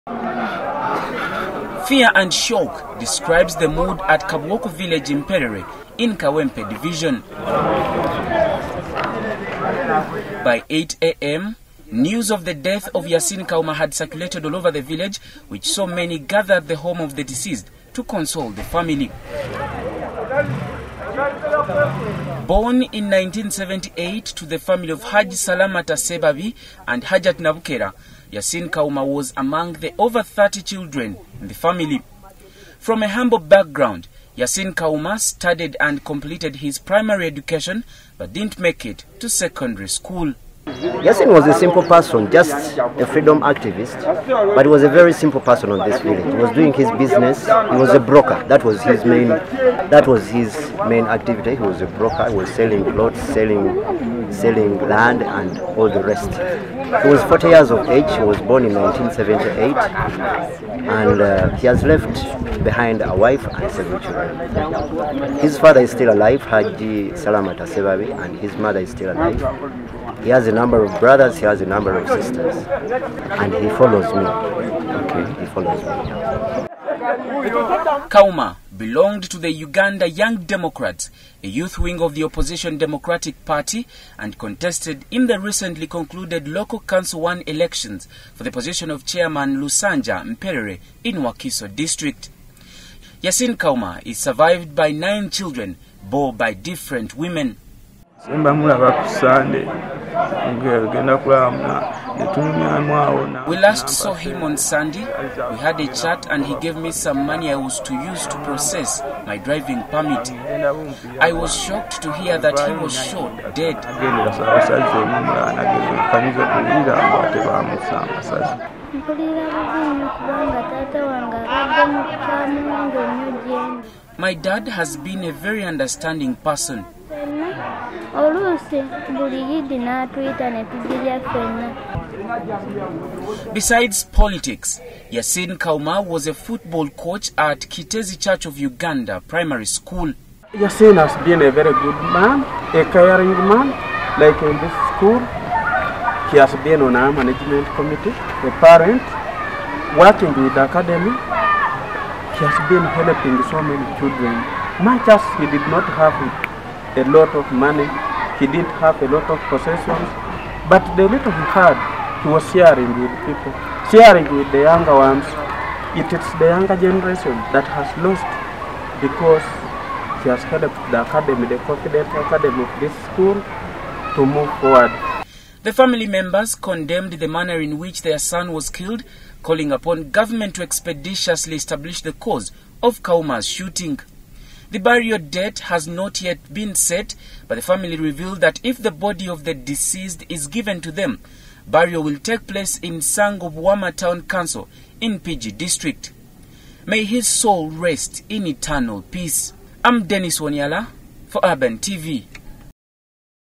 PMLive: Family of the deceased uncertain of where Kawuma's body is
Yasin Kawuma was shot dead yesterday in Arua.